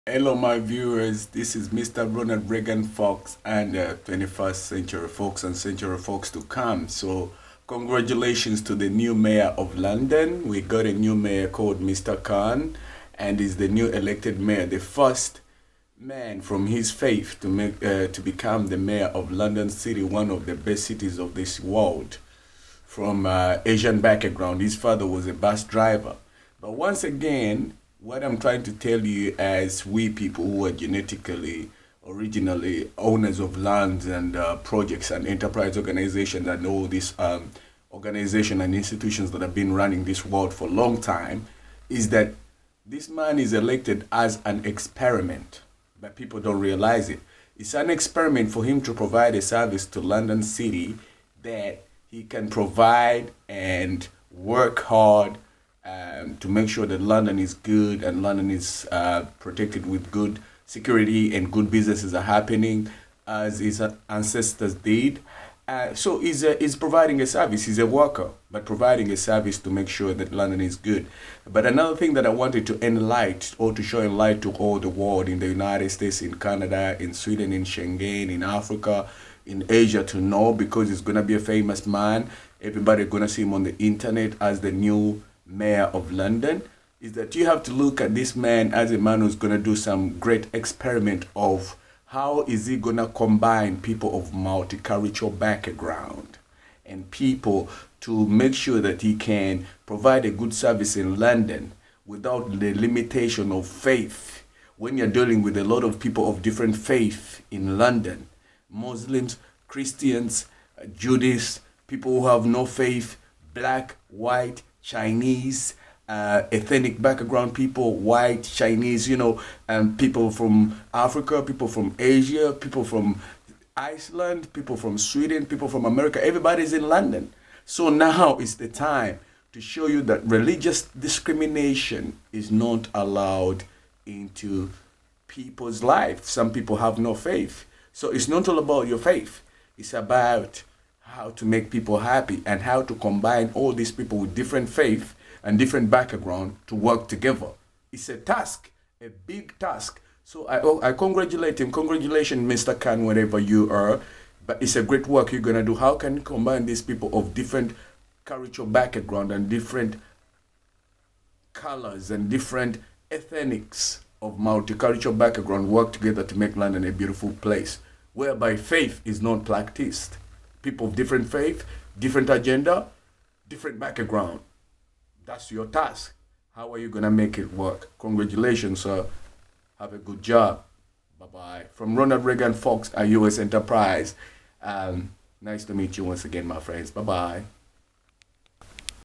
RONALD REGAN FOX SPEECH ABOUT LONDON MOYAR KHAN SADIQ
RONALD  REGAN  FOX SPEECH  ABOUT  THE  NEW  ELECTED  MAOYAR OF  LONDON. THE  EXPERIMENT  IS  A GOOD  IDEA ITS  TIME  TO SEE  HOW  A   MOYAR  CAN  FIGHT  DISCRIMINATION AND  HOW  TO  BRING PEOPLE  TOGETHER  NO  MATTER YOU  FAITH[MULTICULTURAL  IDEOLOGY  FOR  LONDON]